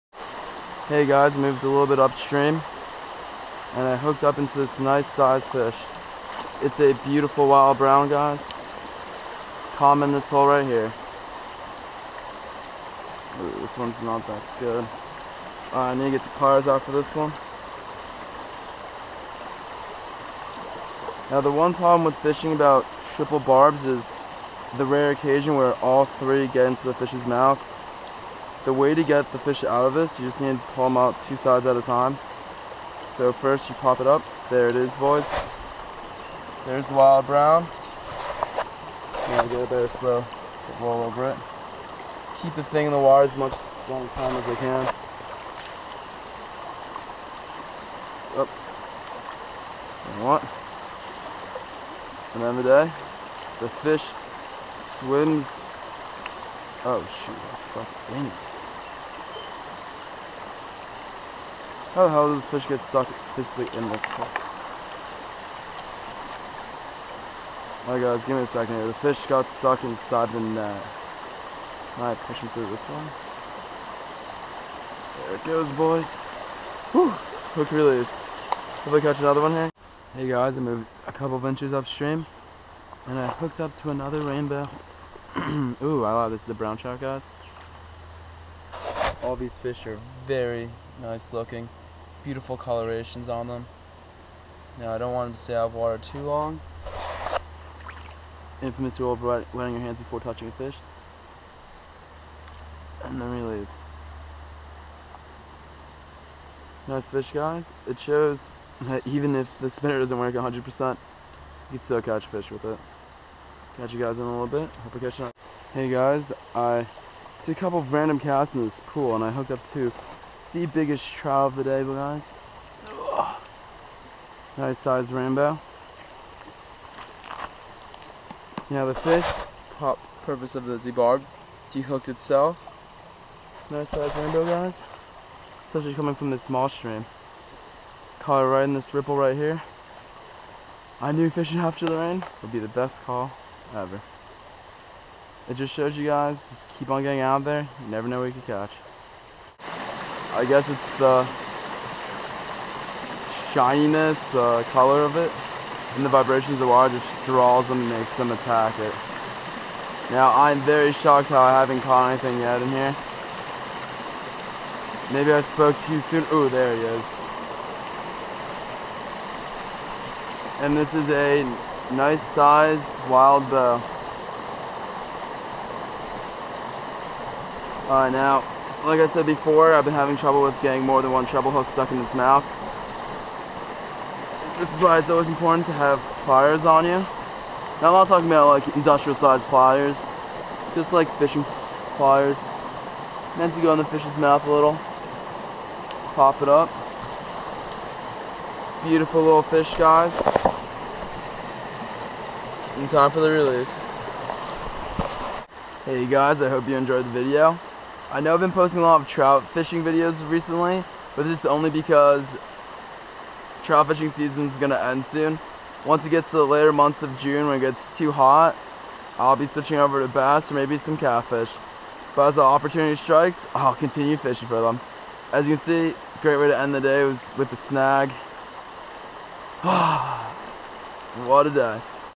Fishing Small Stream for some Wild Trout
This clip was from a fishing trip that I took in early June. It rain the day before, so I had a gut feeling that the fishing would be good.